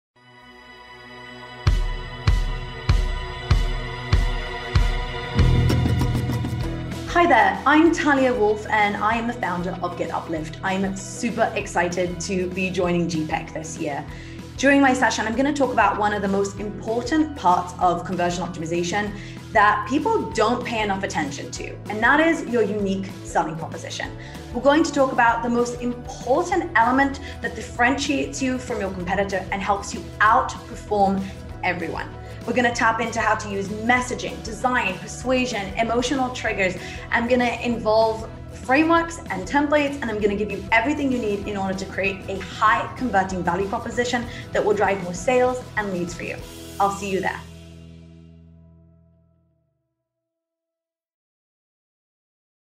Video Talia Wolf speaks at GPeC SUMMIT Online 2020
Talia Wolf - the Founder of GetUplift - joins GPeC SUMMIT Online on November 3rd to talk about how to create a great Unique Selling Proposition, drive more sales and outperform competitors through Conversion Optimization.